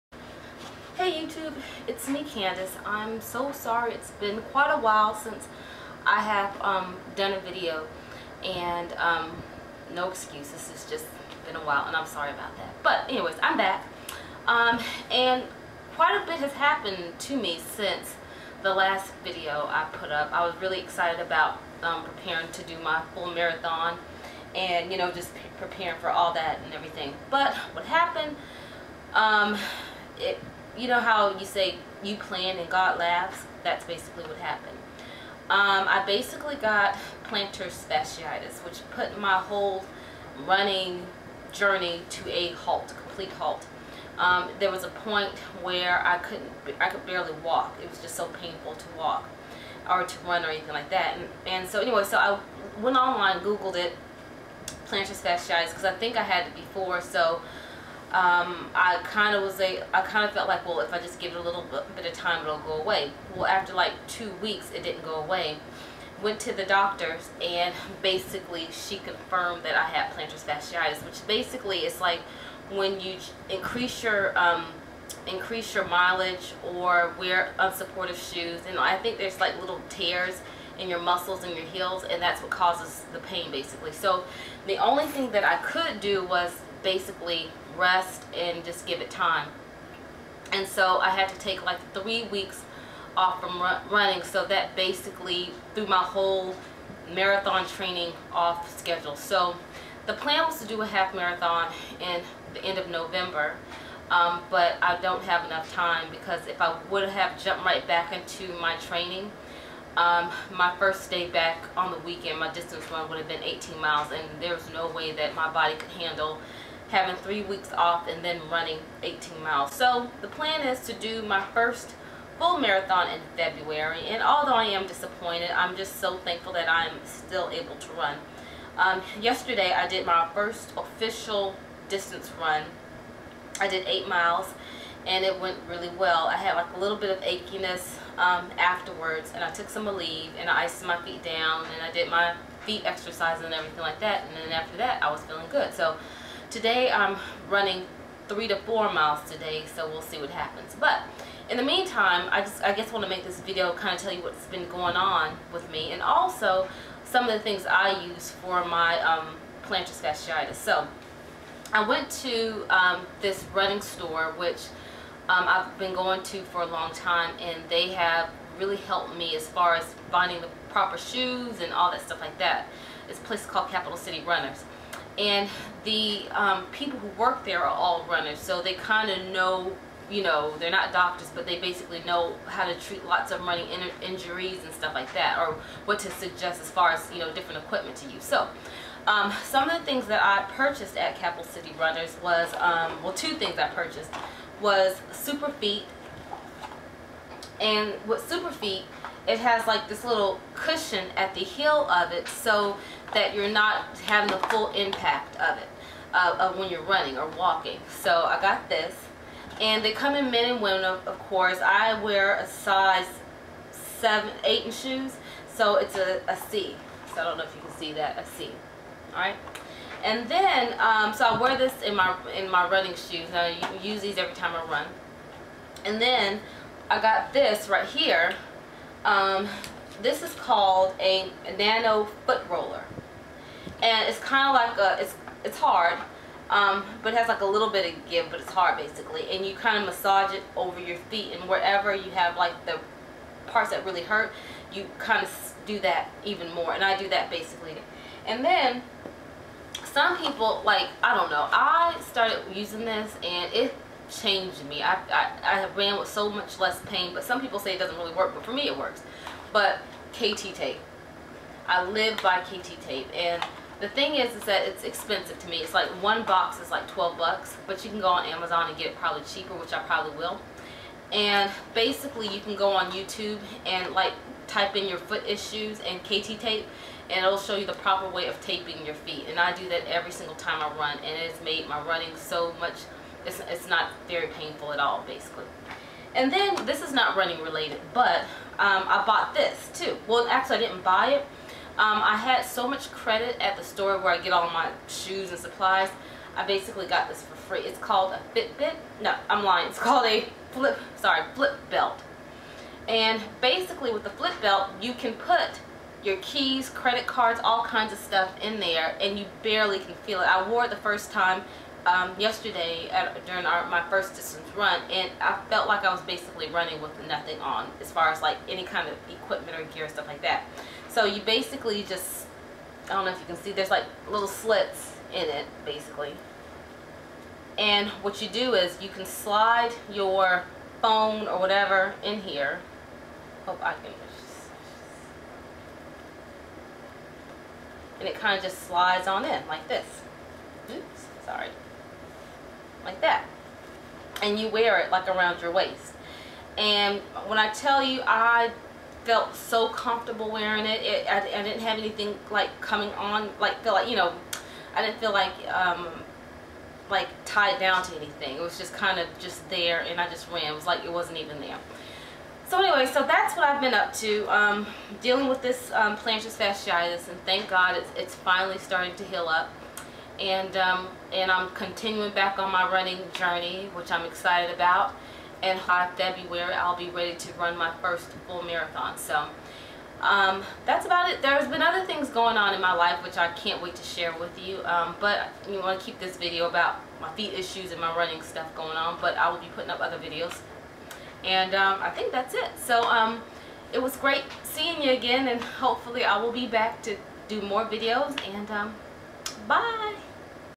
Dealing with my Plantar Fasciitis and Marathon Training
For the last 3 weeks, I have being dealing with my plantar fasciitis.  Here are some of the things I used to help  me run again.